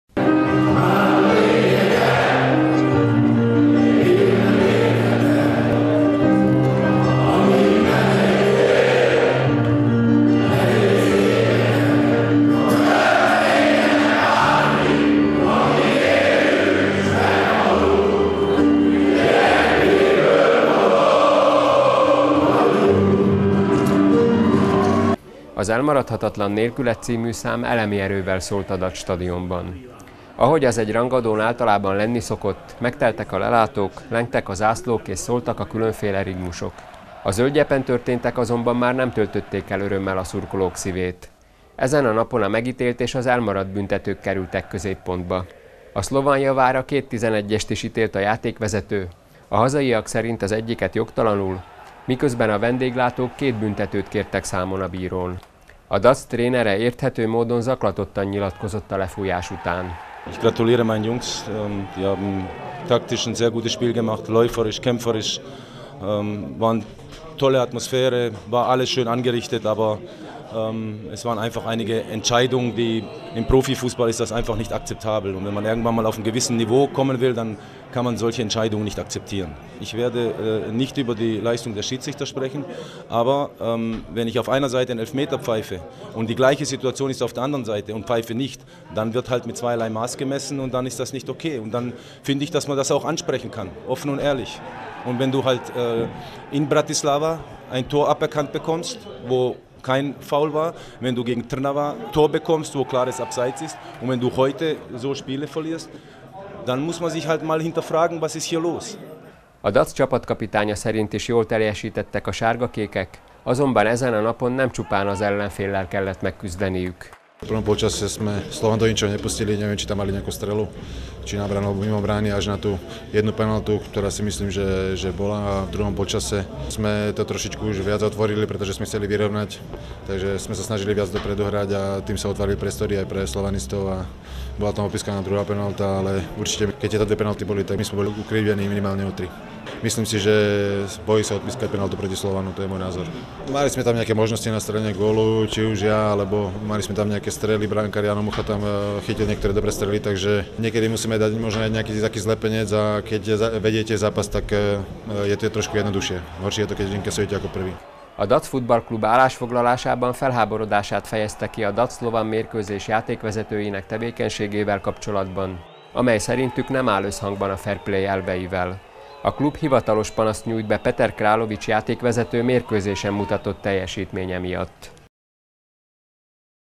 Očakávané derby DAC-Slovan v hlavnej roli s rozhodcom
V 25. kole Fortuna ligy sa hralo očakávané derby stretnutie DAC-Slovan. K zvýšenému očakávaniu prestížneho zápasu zodpovedal aj počet divákov. Hralo sa pred plným štadiónom, čo z dôvodu prestavby futbalového stánku v súčasnosti znamená 5200 divákov. Derby vďaka dvom gólom z pokutových kopov vyhrali belasí v pomere 2:0. Počas 90 minút však v centre pozornosti neboli iba hráči na ihrisku. Na rozhodcu zápasu sa pre diskutabilné verdikty upriamila tiež veľká pozornosť.